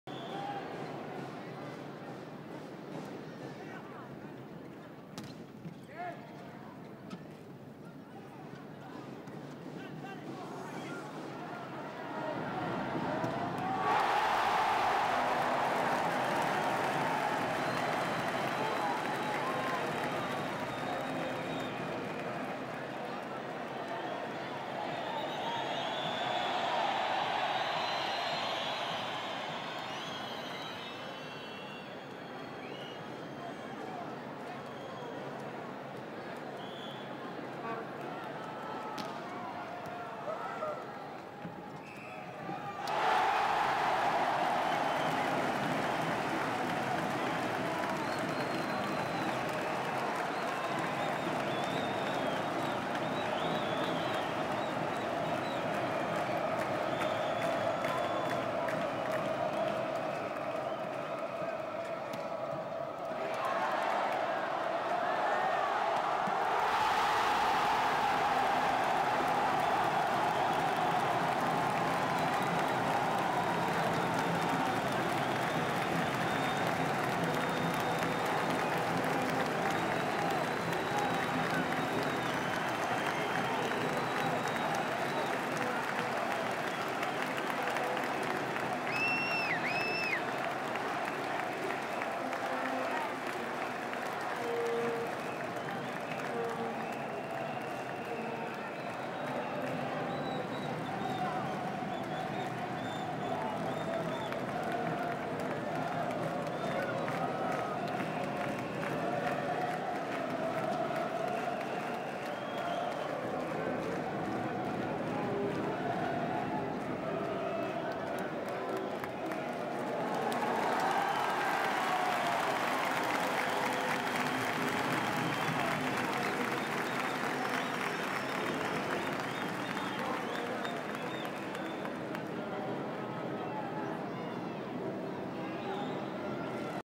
AV-3387 [Argentina derrota a México en Mundial de Vóley 1982] (fragmento)
Partido disputado entre Argentina y México, en el marco del Campeonato Mundial de Vóley de 1982. El tercer puesto alcanzado en aquel Mundial marcaría el hito más alto del vóley argentino. Le valió al grupo dirigido por el coreano Young Wan Sohn pasar a la historia como la “generación dorada” del vóley. En esta segunda presentación del equipo argentino en el torneo, la dura selección mexicana pelea cada punto dándole dramatismo a la victoria argentina, mientras baja un aliento incesante de las tribunas.